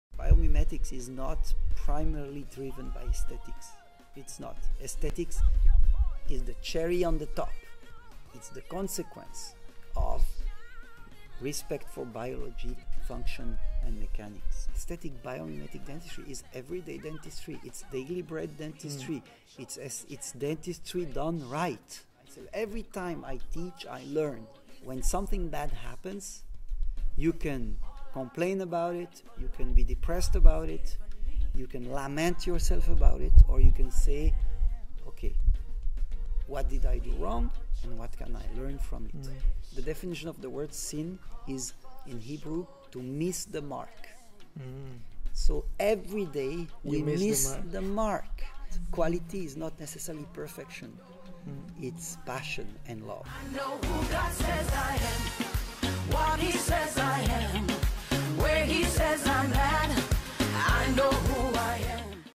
Interview with Dr. Pascal Magne-Godfather of Biomimetic Dentistry [TEASER]
A teaser for the interview with Dr. Pascal Magne, the Godfather of Biomimetic Dentistry.

During Dr. Pascal Magne's training in Kunkela Academy, Czech Republic.

THE FULL INTERVIEW IS COMING SOON...

Video Credits: Ufuk Ozcan